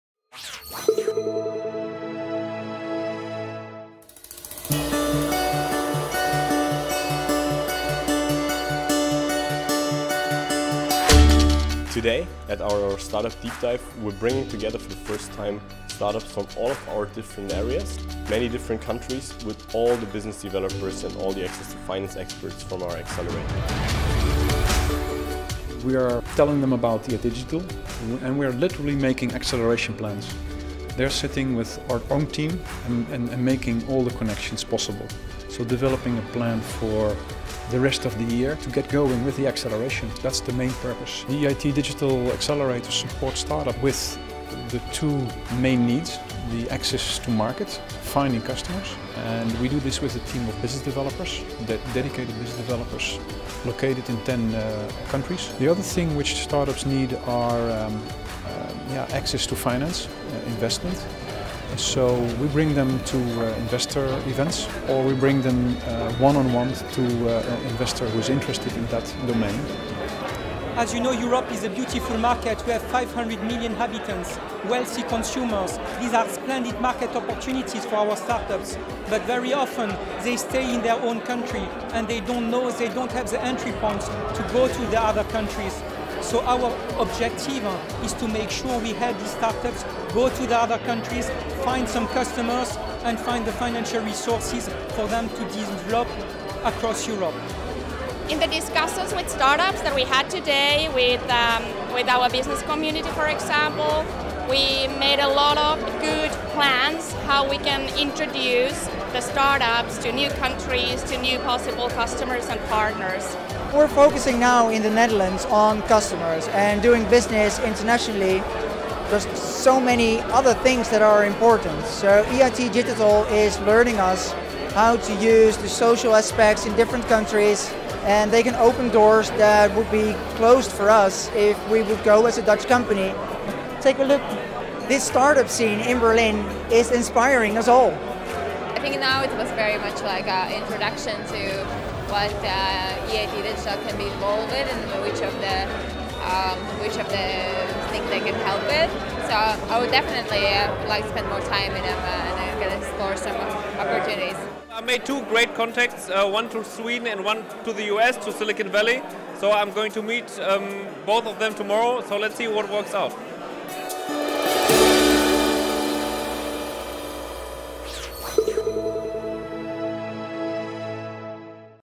EIT Digital Accelerator - Scaleups Welcome
In February 2016, more than 40 business developers and financing experts from all across Europe came together to welcome 23 new startups in the EIT Digital ecosystem. Most of the startups were winners from the EIT Digital Challenge 2015. If you are a startup ready to scale internationally and want to take part as well, apply here for the EIT Digital Challenge 2016